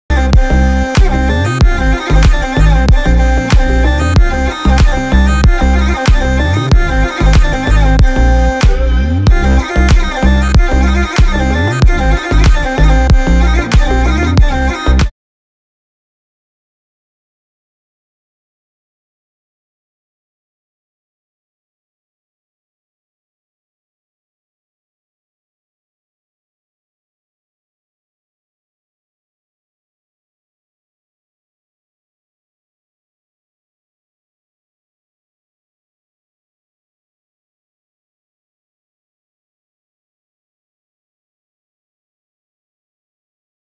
Today Review Amazon Plastic Cake Measuring Cup.
INOVERA (LABEL) Plastic Measuring Cups and Teaspoon Tablespoon Spoons Tools Set for Cake Baking and Cooking (Sky Blue) - 12 Pieces